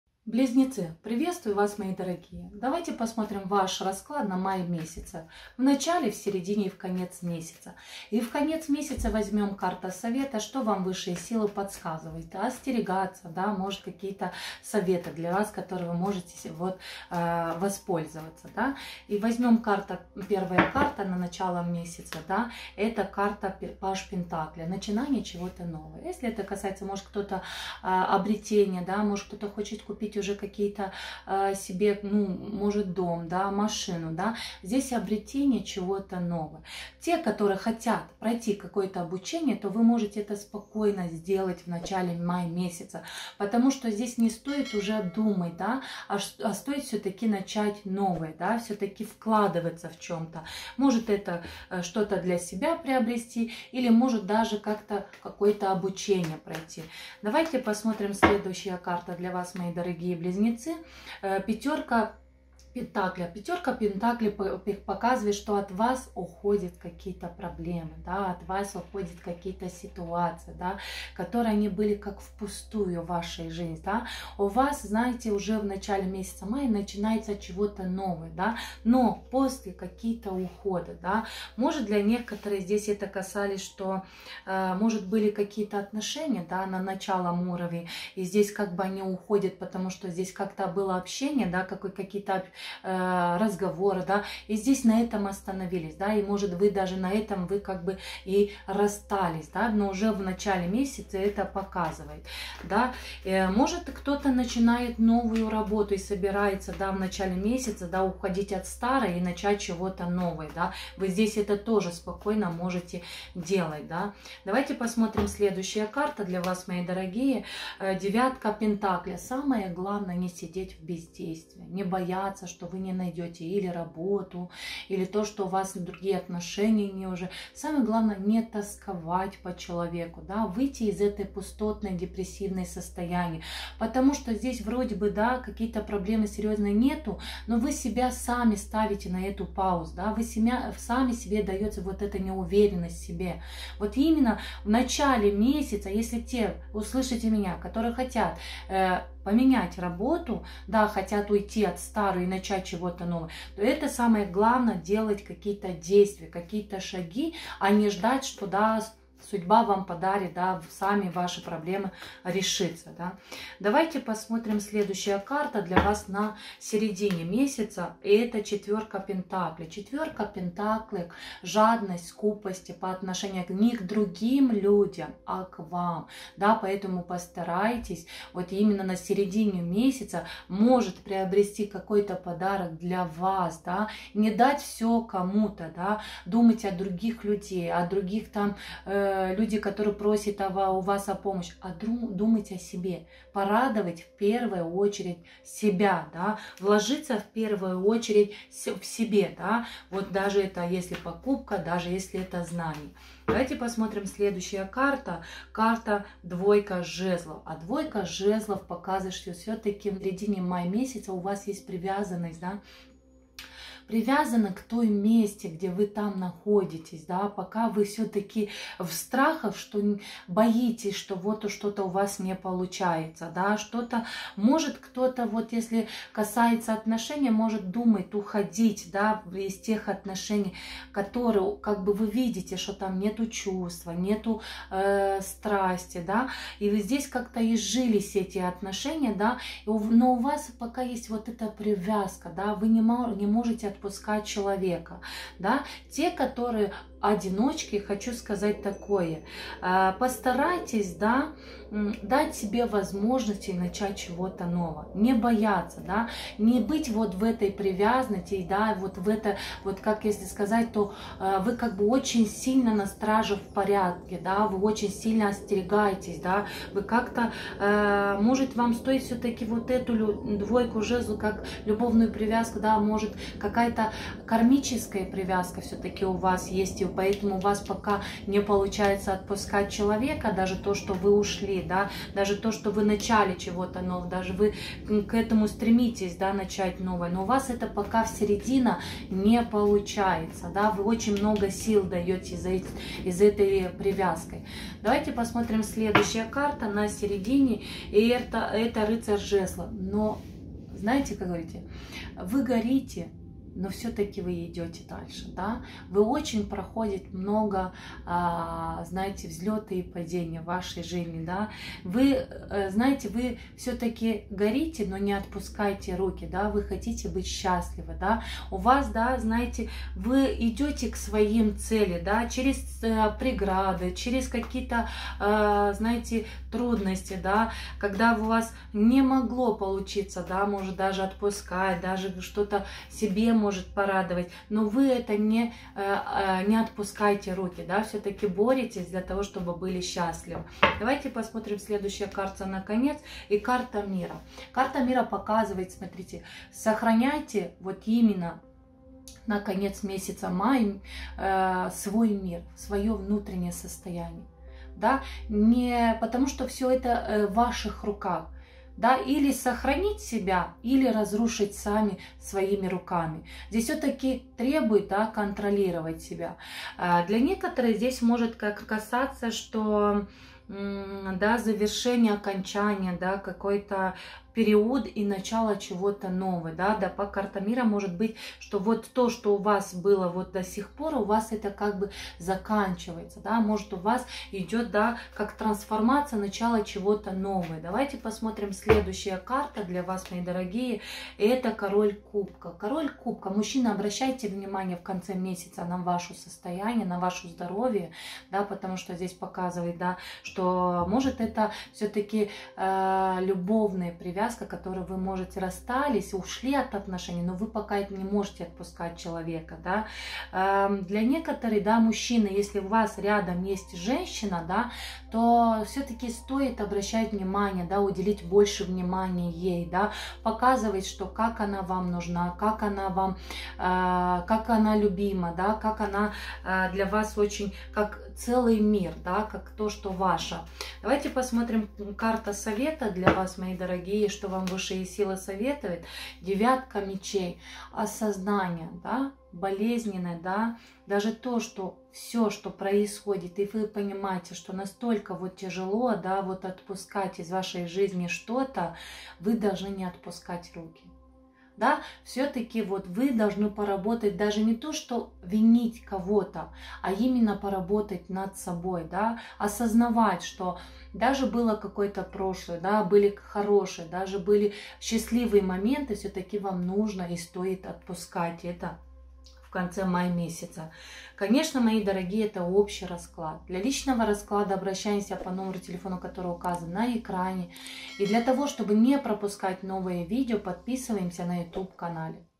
БЛИЗНЕЦЫ МАЙ 2024. Таро гороскоп для БЛИЗНЕЦОВ на МАЙ 2024 от Доины Римари
Что ждет БЛИЗНЕЦОВ в МАЕ 2024 года?